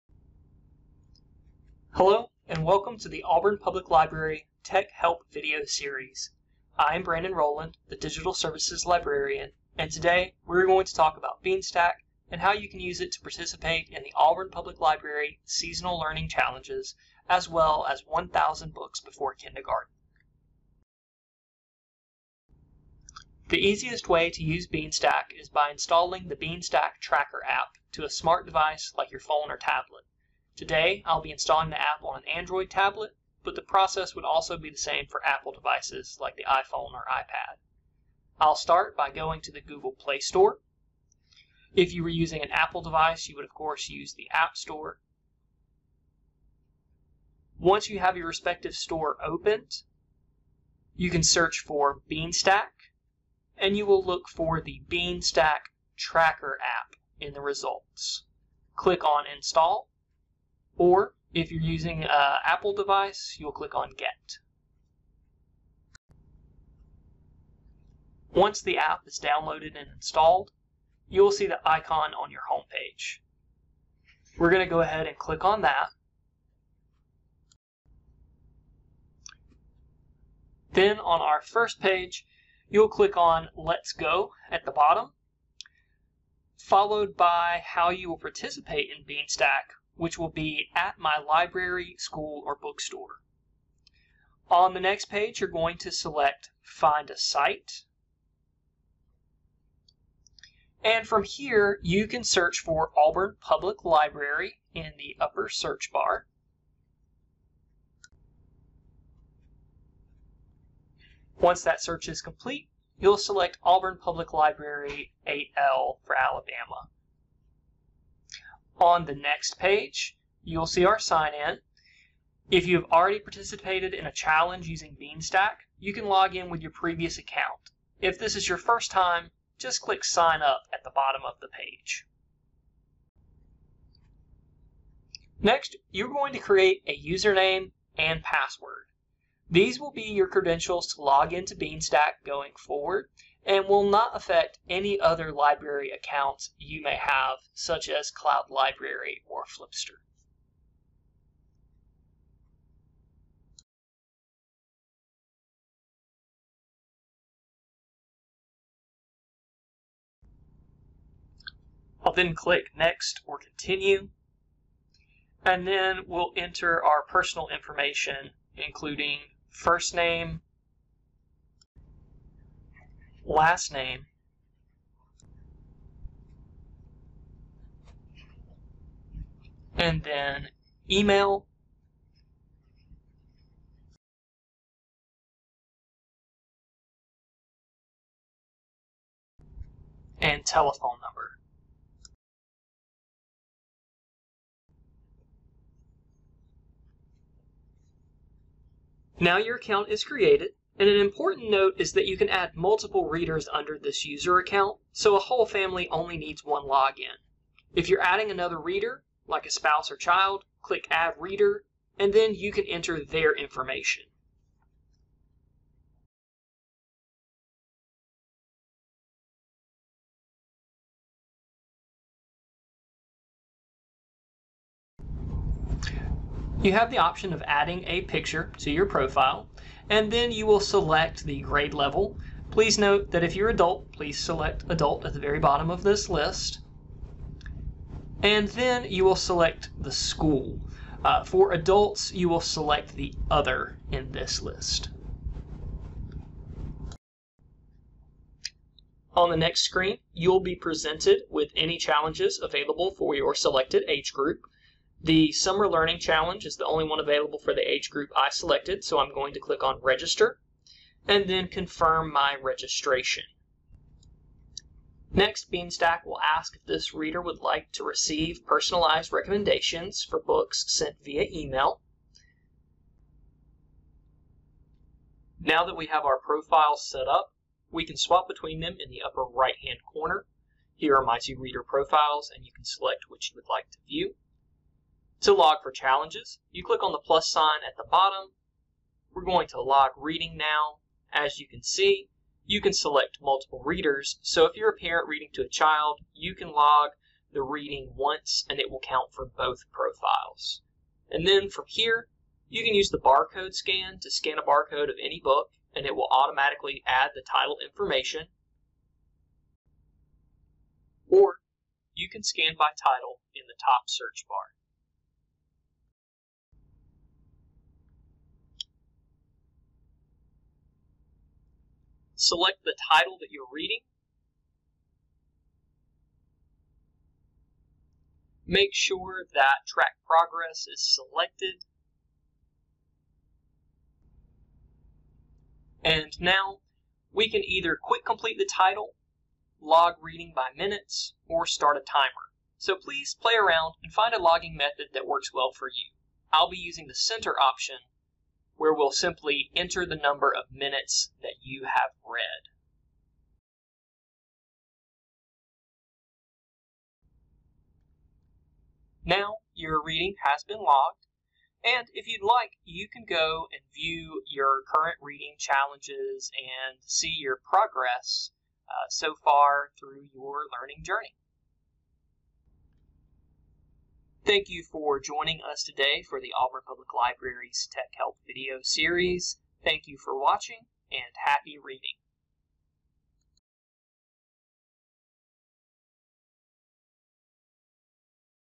Auburn Public Library Tech Help: Beanstack
Digital Services Librarian Brandon Rowland walks you through how to use the Beanstack reading tracker app.